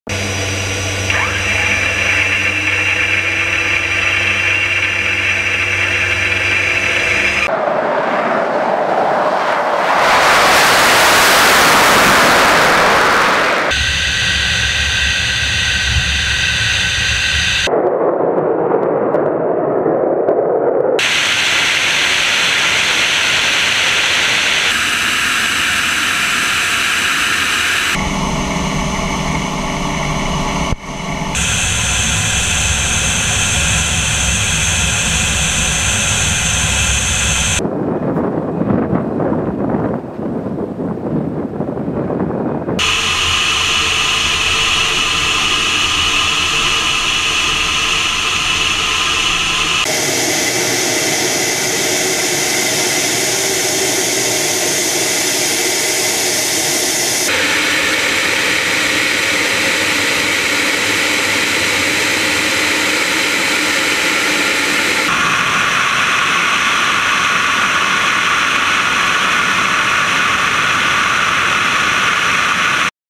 Su-25 acrobatic training flight in the Eastern Military District
Групповой пилотаж летчиков штурмового авиационного полка ВВО